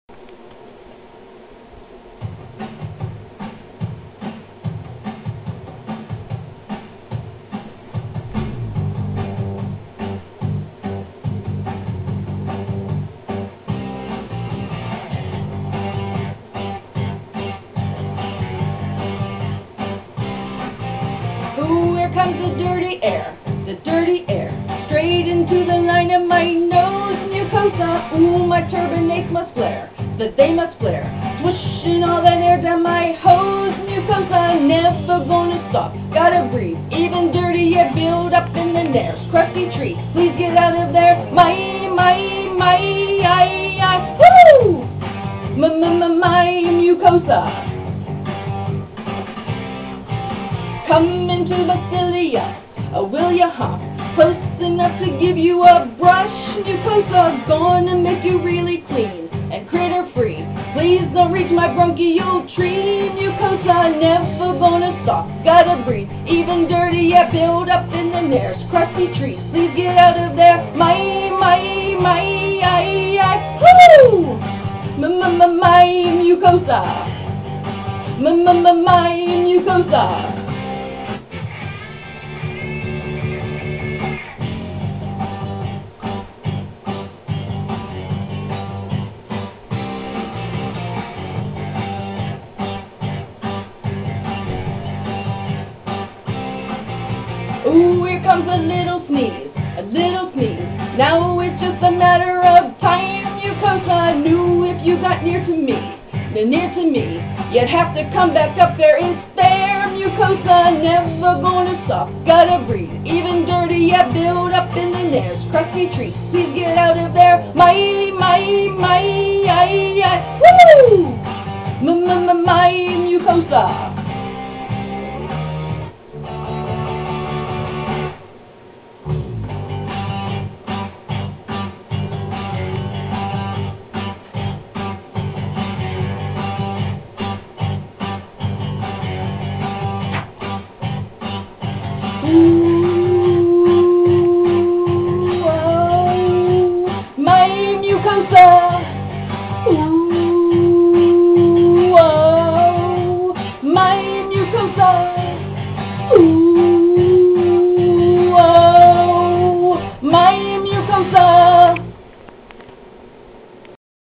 My Mucosa-- Pam: Mom and Student CRT
Repiratory Therapist explaining mucosa